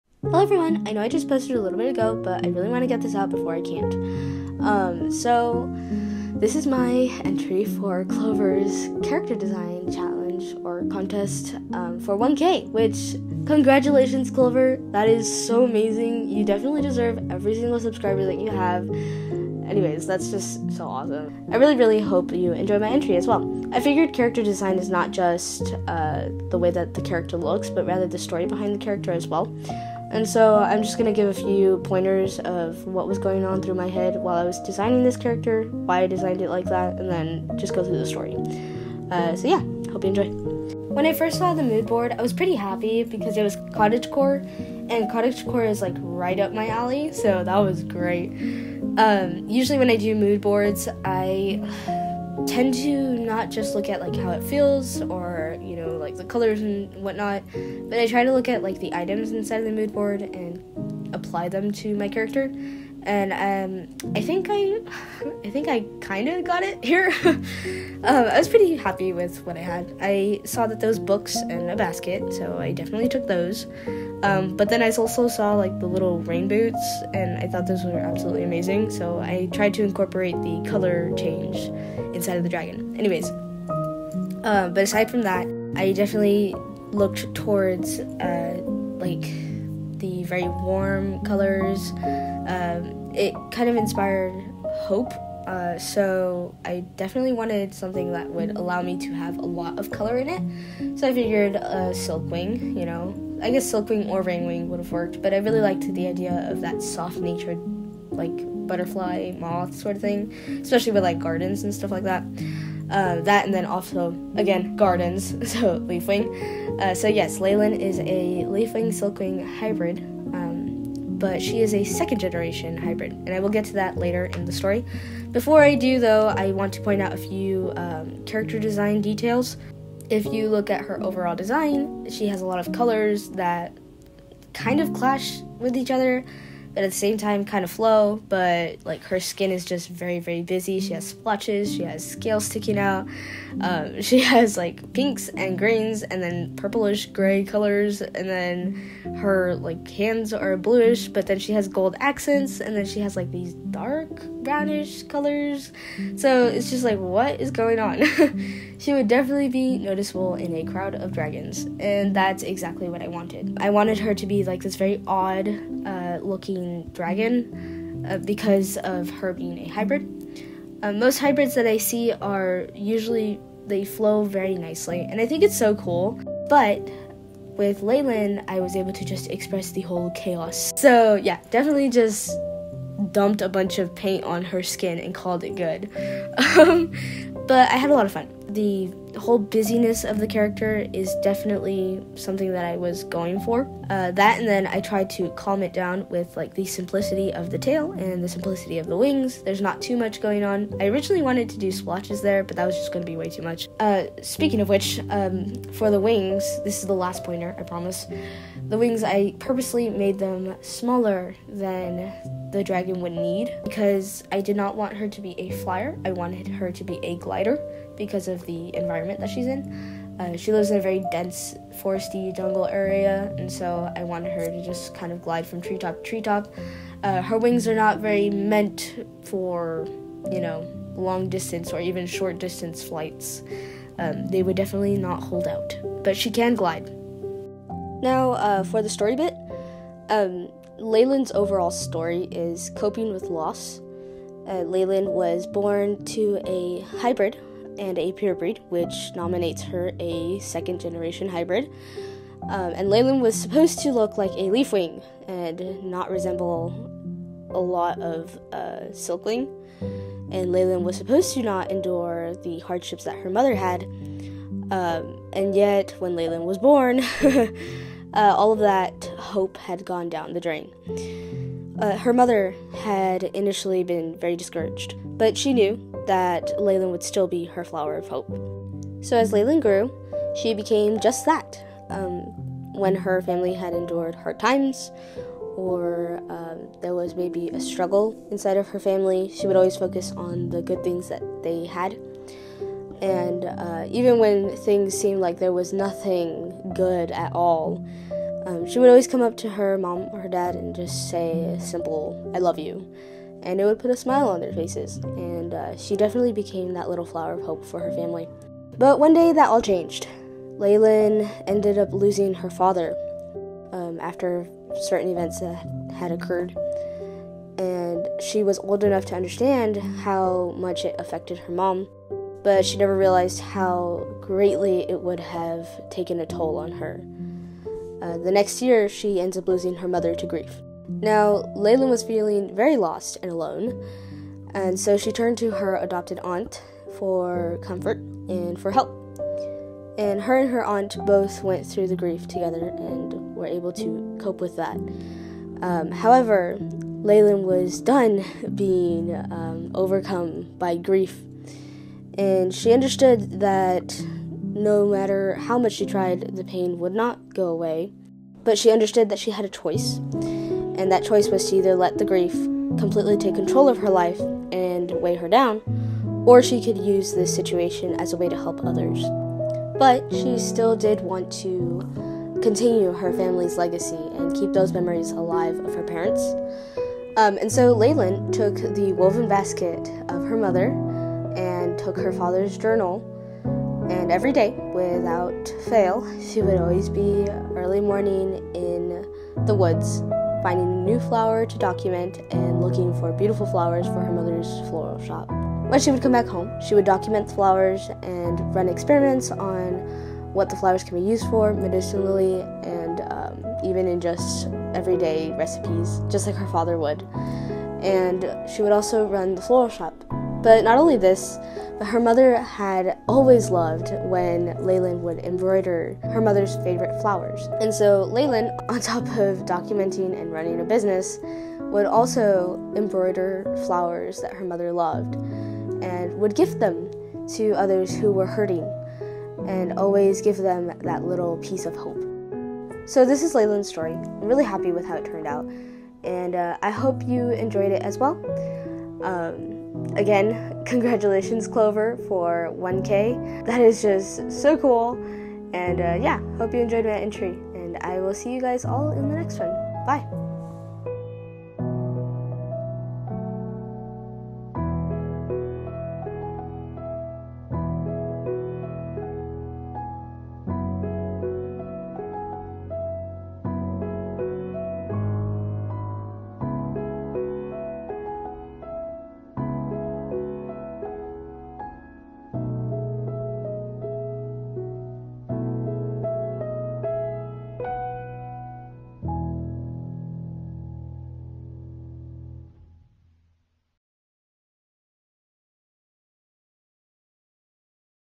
My Entry for @Not_coraline 1K Character Design Contest!
Hellooooo! If @Not_coraline you are watching this, CoNgRAtS! Been following your channel for a while now, and it’s cool to see how fast you’re channel has grown. Hope you enjoy my entry! :D